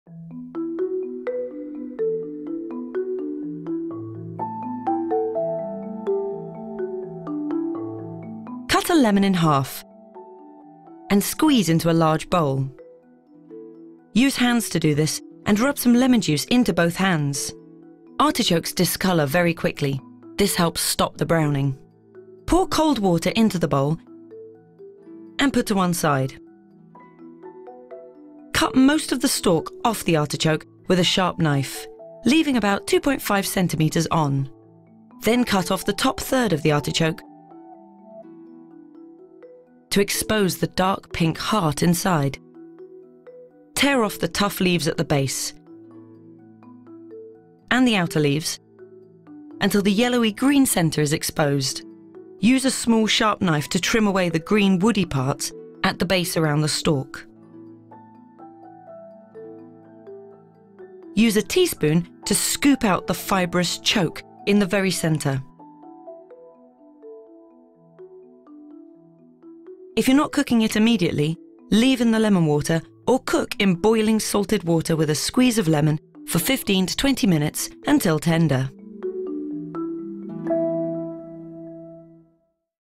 How To Prepare Fresh Artichoke Hearts | Good Housekeeping UK
Knowing how to prepare fresh artichoke hearts can seem fiddly, but our step-by-step video will show you how to do it easily.

1. Put lemon juice and cold water into bowl and set aside. Cut most of stalk off artichoke with a sharp knife, leaving about 2.5cm (1in) on. Cut off top third of artichoke, to expose dark pink ‘heart’ inside.

2. Tear off tough leaves at base and outer leaves until yellowy, green centre is exposed.

3. Use a small sharp knife to trim away green, woody parts at base around stalk.

4. Use a teaspoon to scoop out fibrous ‘choke’ in centre. If you’re not cooking it immediately, leave in lemon water or cook in boiling salted water with a squeeze of lemon for 15-20min until tender.

Find out how to prepare fresh artichoke hearts on the Good Housekeeping